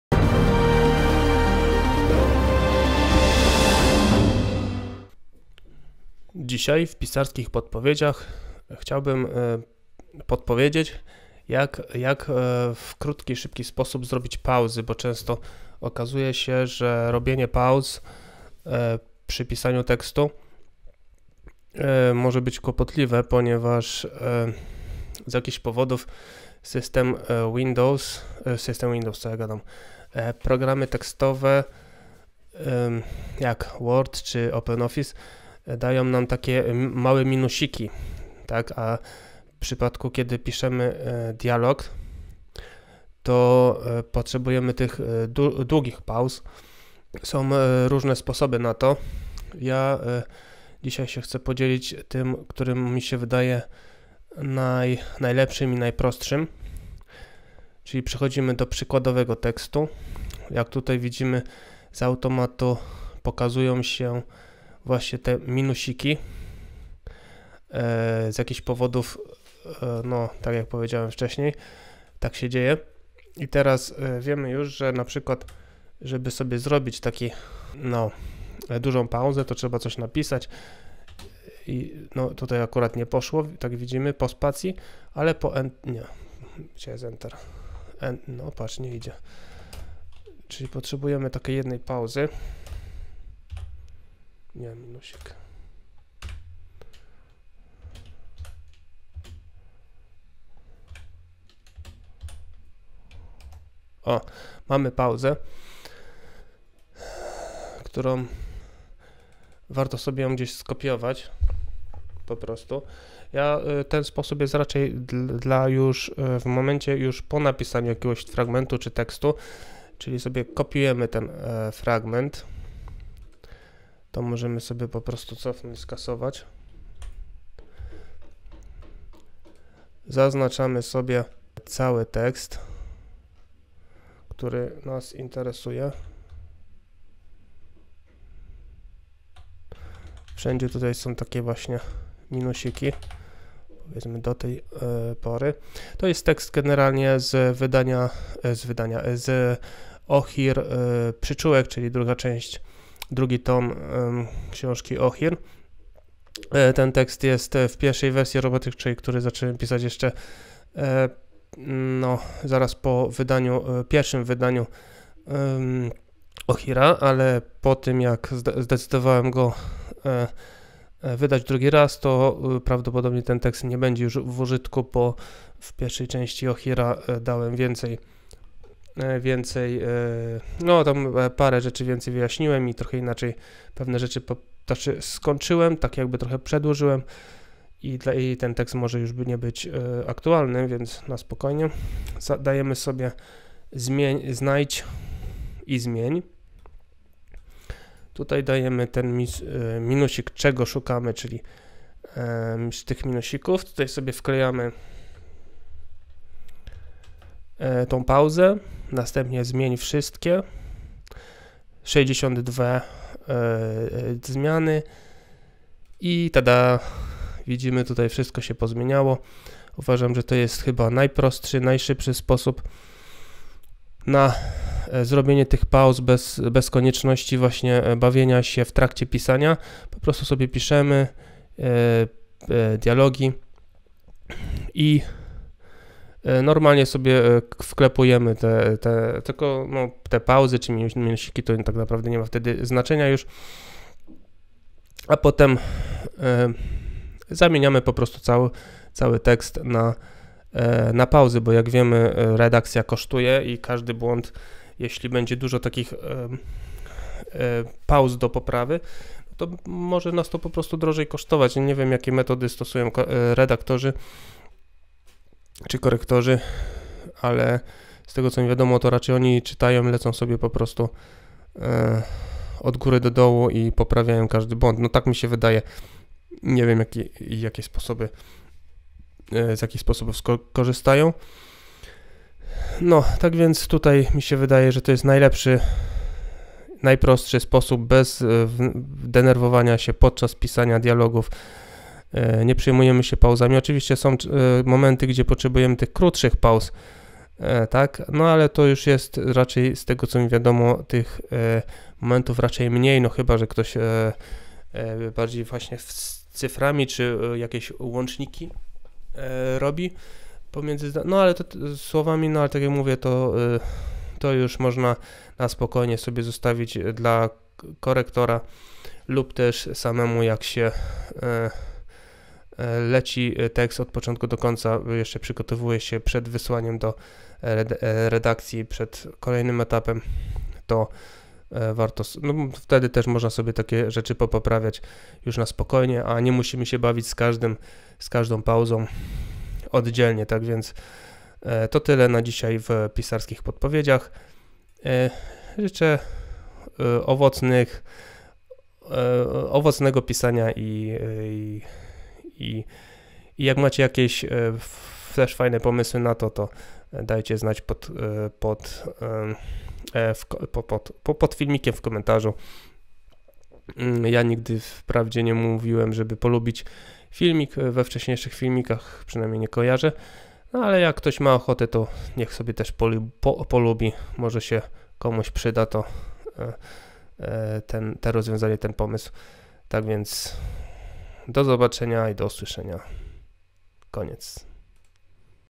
Pauzy dialogowe? Jak zrobić je sprawnie i szybko?
Pauzy, półpauzy, łączniki, minusy i myślniki.
W tym odcinku podpowiadam jak szybko i sprawnie można poradzić sobie z pauzami w programach tekstowych tj. openoffice czy word.

Podczas pisania dialogów zastosowanie właściwego znaku bywa uciążliwe.

Mój sposób rozwiązuje problem martwienia się skąd wziąć pauzę do dialogu.